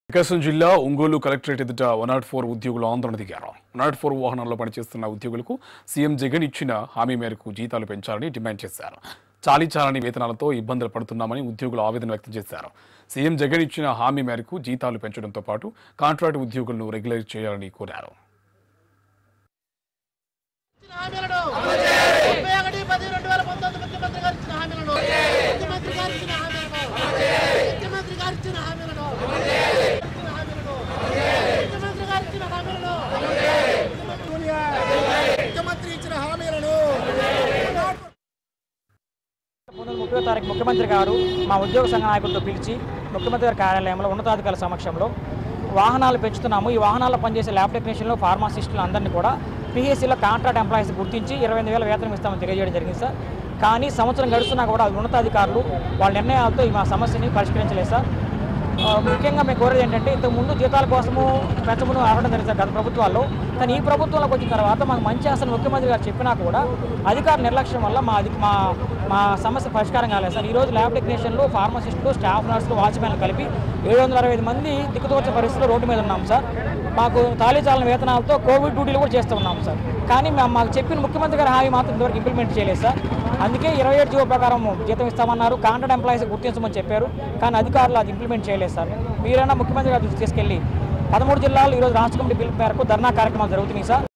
104 Employees Protest For Salaries Hike at Ongole Collectorate | iNews
104 Employees Protest For Salaries Hike at Ongole Collectorate | iNews